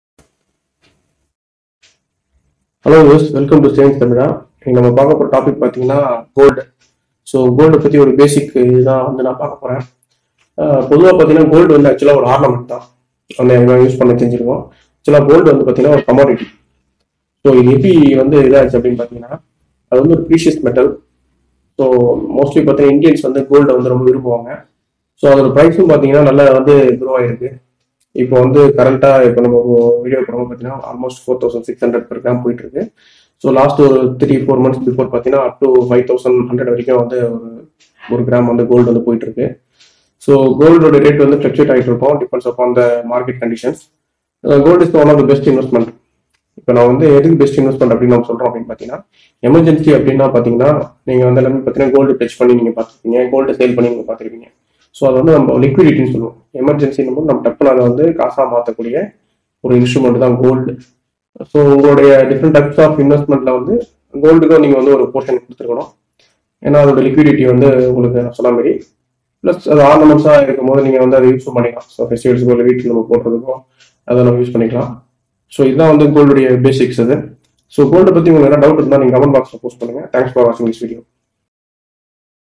Importance Of Gold | தங்கத்தின் முக்கியத்துவம் | 2021 | SAVINGS TAMIZHA | IN TAMIL
GOLD:

Of all the precious metals, gold is the most popular as an investment. Investors generally buy gold as a way of diversifying risk, especially through the use of futures contracts and derivatives. The gold market is subject to speculation and volatility as are other markets. Compared to other precious metals used for investment, gold has been the most effective safe haven across a number of countries.

Gold has been used throughout history as money and has been a relative standard for currency equivalents specific to economic regions or countries, until recent times. Many European countries implemented gold standards in the latter part of the 19th century until these were temporarily suspended in the financial crises involving World War I. After World War II, the Bretton Woods system pegged the United States dollar to gold at a rate of US$35 per troy ounce. The system existed until the 1971 Nixon Shock, when the US unilaterally suspended the direct convertibility of the United States dollar to gold and made the transition to a fiat currency system. The last major currency to be divorced from gold was the Swiss Franc in 2000.

Since 1919 the most common benchmark for the price of gold has been the London gold fixing, a twice-daily telephone meeting of representatives from five bullion-trading firms of the London bullion market. Furthermore, gold is traded continuously throughout the world based on the intra-day spot price, derived from over-the-counter gold-trading markets around the world (code "XAU"). The following table sets out the gold price versus various assets and key statistics at five-year intervals.